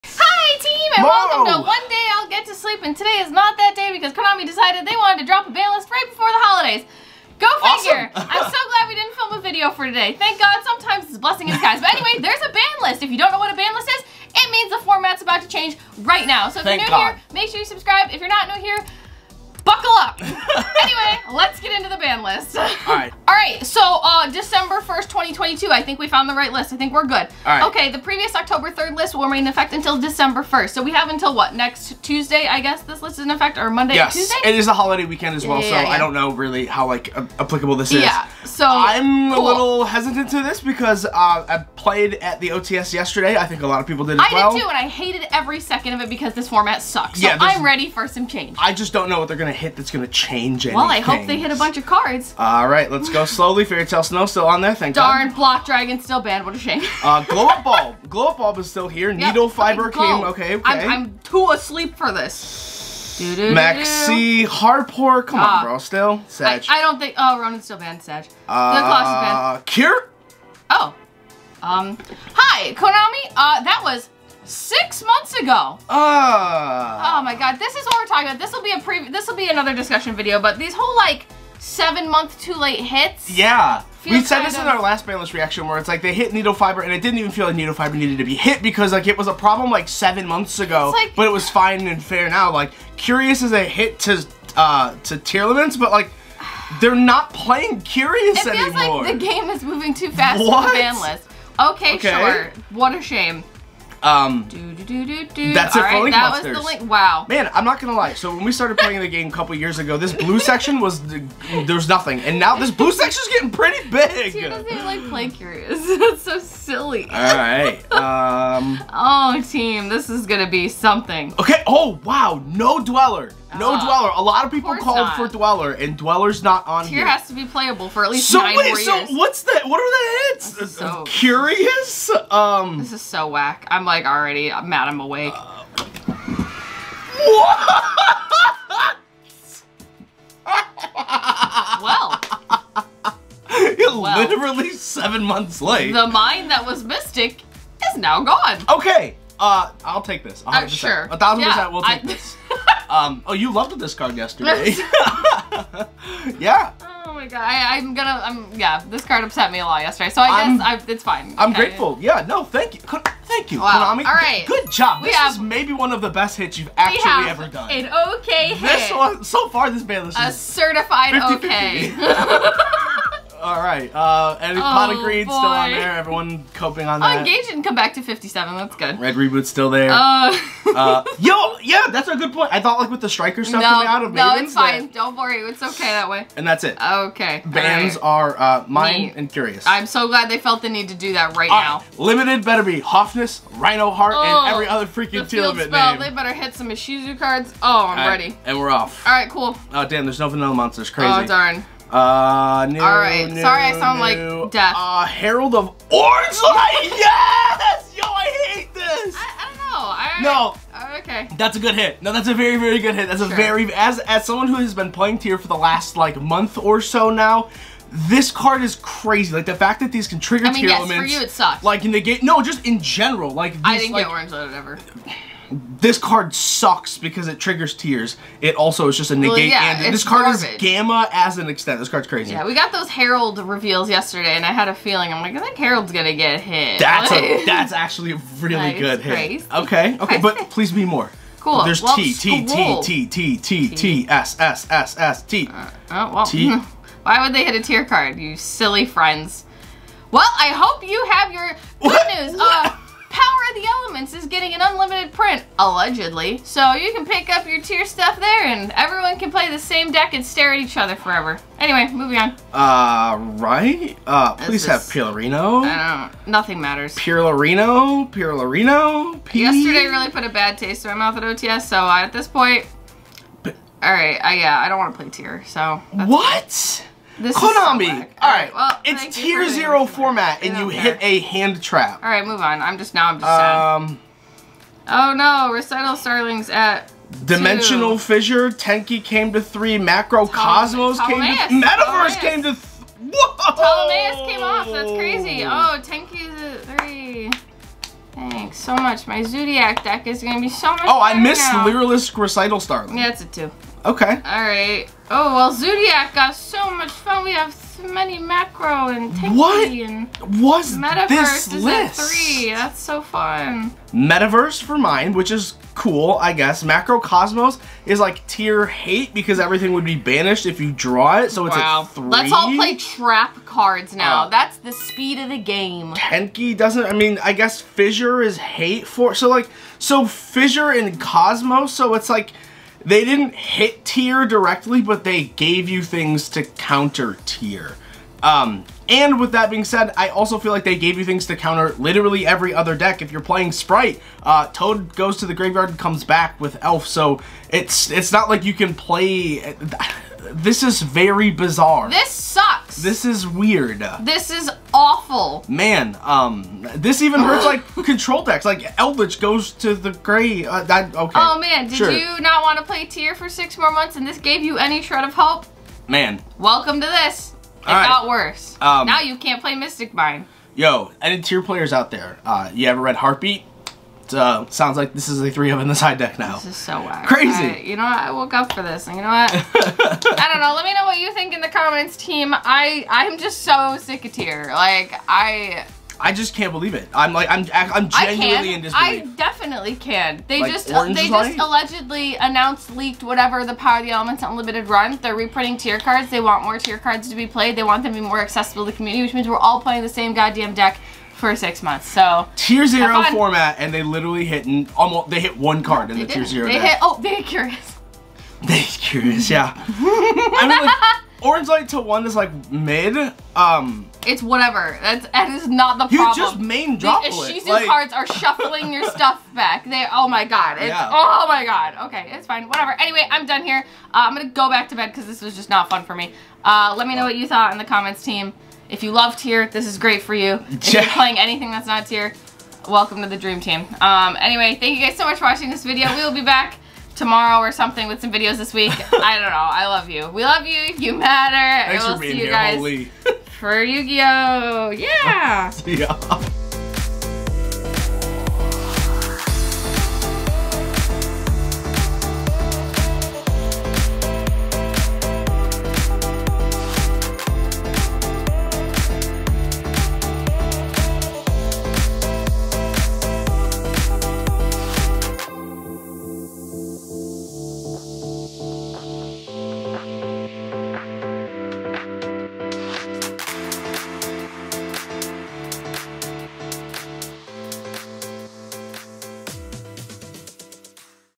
ITS A NO FOR ME DOG. | Yu-Gi-Oh! Banlist Reaction Nov. 2022
🌟GET ALL YOUR YU-GI-OH PRODUCTS FROM BIG 3 DEALS!!🌟

 ~ Codes May Be Case Sensitive ~